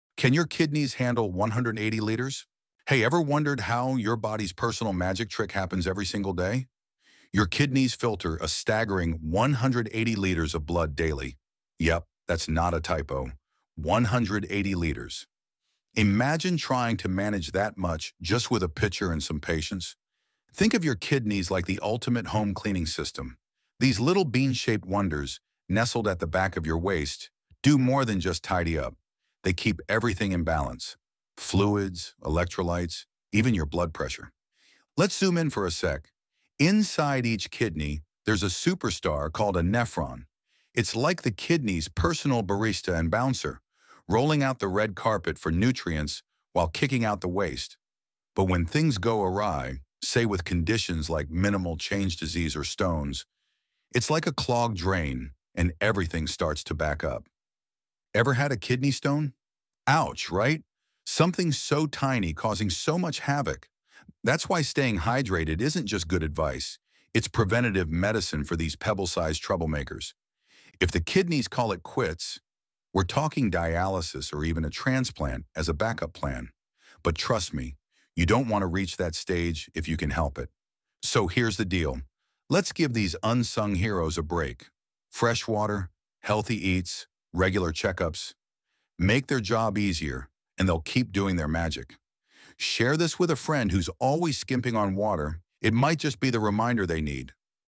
Unraveling Kidney Wonders: 180 Liters of Magic! ✨🧠
Did you know your kidneys filter 180L of blood daily? 🤨 They're crucial for keeping you in balance! Let’s protect these little powerhouses with hydration and healthy habits. Share this with a friend who occasionally forgets their H2O! 💧